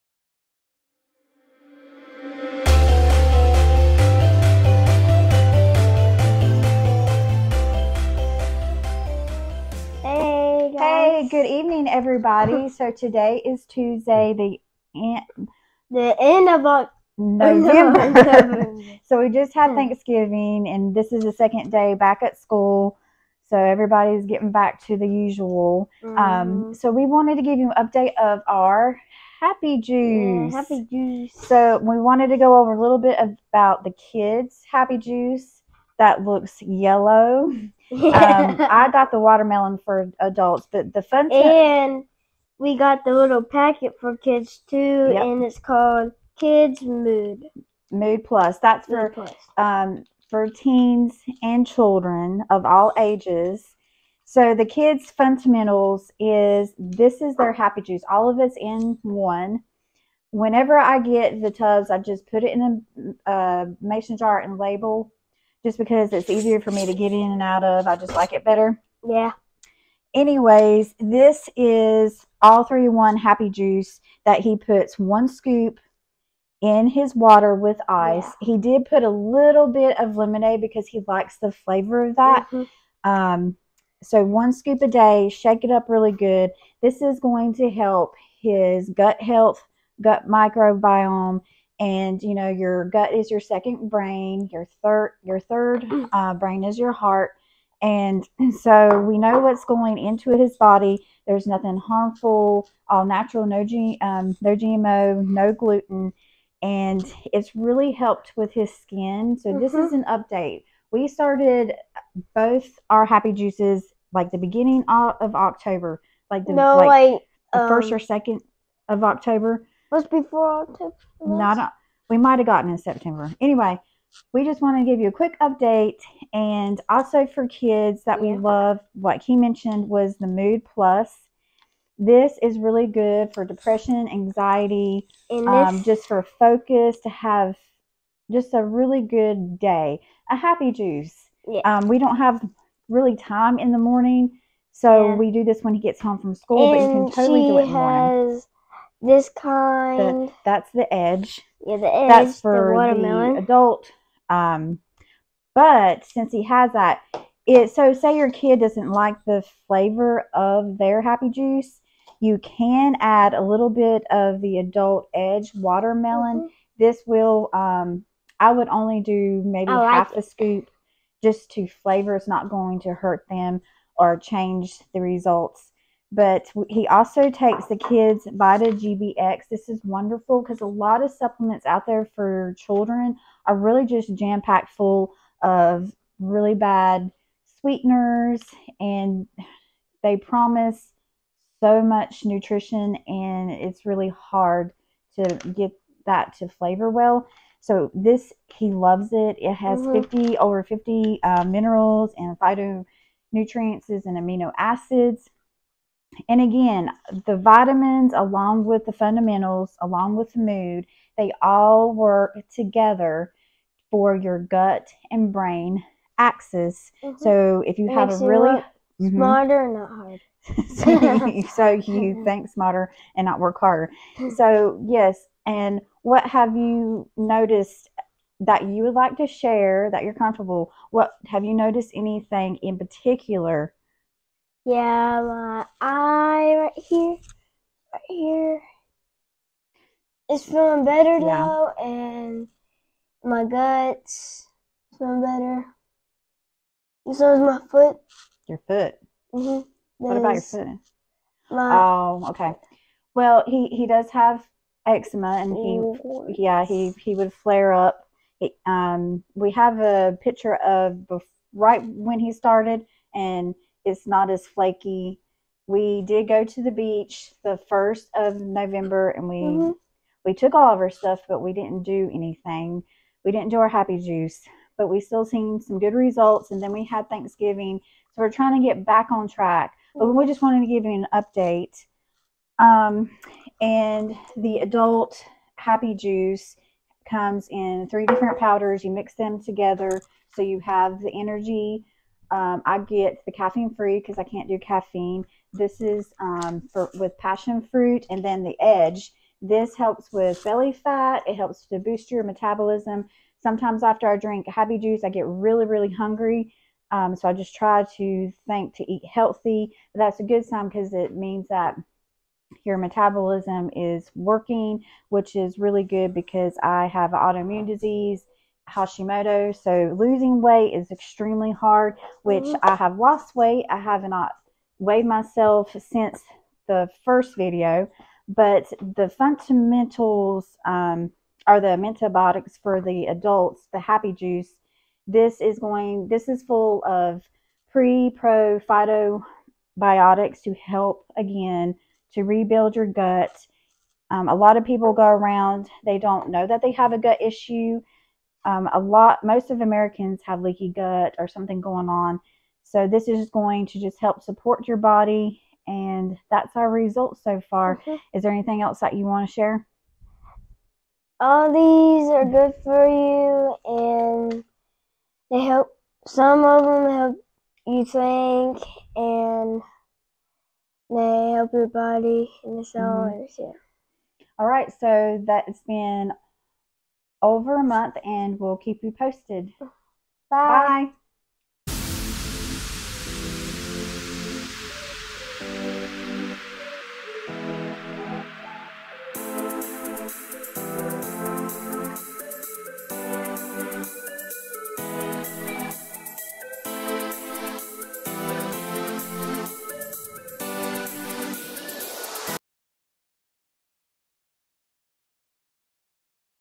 Update on our Happy Juice/Gut Health!
2nd video on our Happy juice products. We have seen some good results so far. Started early October. Hunter has noticed quite a change in his bathroom routine and the way his stomach feels. He loves the way he has happy thoughts throughout the day. His eczema has really improved!! Any skim related issue related to your gut;) funny he said that he has been having more gas. That’s actually a great sign to show the bad bacteria in the gut is dying off.

I have noticed I’m less swollen. Granted just went on vacation and thanksgiving (I’ve Indulged) 😂 still less Edema then If not been startinn products. Getting back to our regimens. Doing another 3 day reboot cleanse. Started Monday to help flush my body out. I also still have more energy and just feel great after I drink happy juice. I have never trouble with “going” it’s just more smooth 🤭😬
I have lost weight… keep ya posted exactly how much.

Here is my link to save $10 off and get started for yourself! Money back, no required monthly orders, clinically proven.. nothing to loose but negative thoughts!